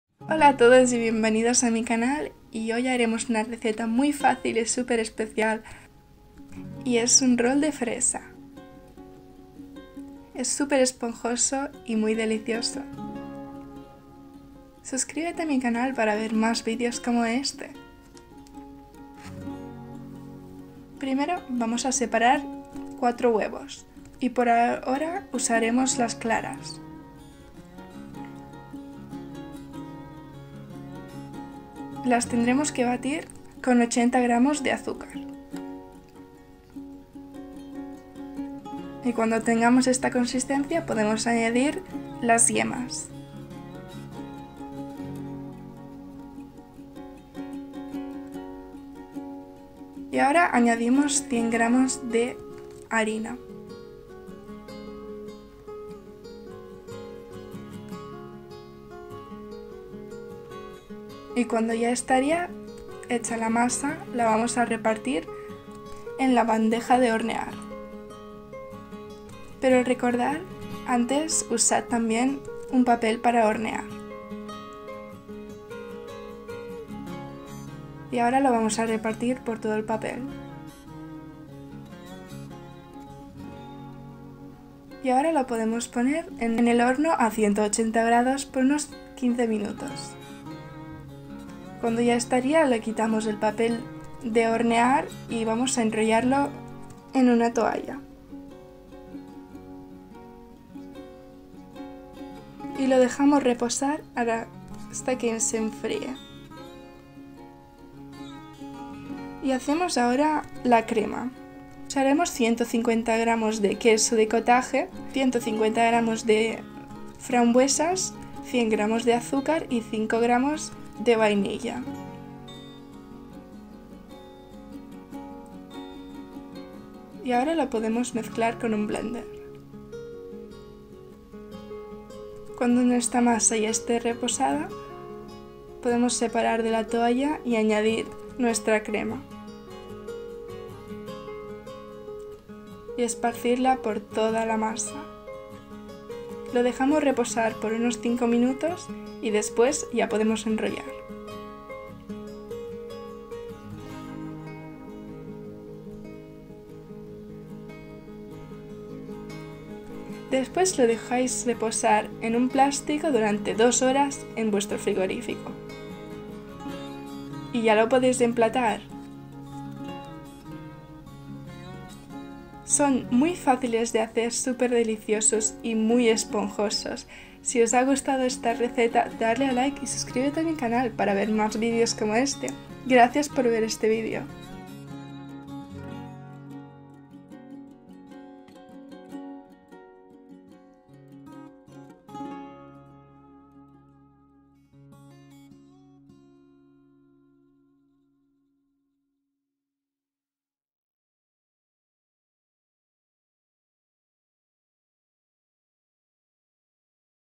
ROLLO muy simple para el té. Comida casera
Un panecillo muy sencillo para el té. Crema ligera de galleta y cuajada de fresa. Es fácil de preparar. Las fresas se pueden sustituir por bayas o frutas. Asegúrate de cocinarlo, está delicioso.

INGREDIENTES:
Galleta:
huevos - 4 piezas
Relleno:
fresas - 150 g
requesón - 150 g
azúcar - 50 g
azúcar de vainilla - 5 g

On this cooking channel you will find detailed recipes that you can easily make at home.